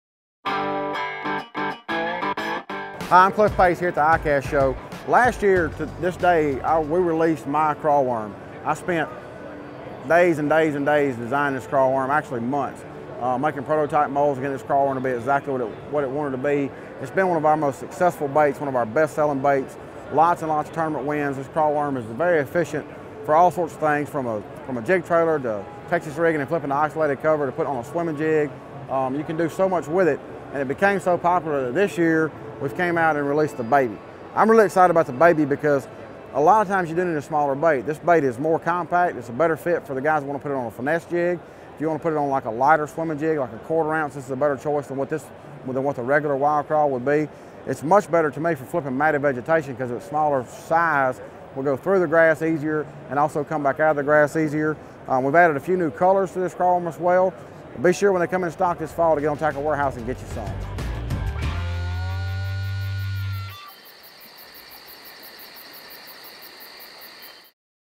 V&M Wild Thang Craw 3.5" with Cliff Pace | ICAST 2015
Tackle Warehouse discusses the new V&M Baby craw Worm with Elite Series Pro Cliff Pace at the 2015 ICAST Show in Orlando.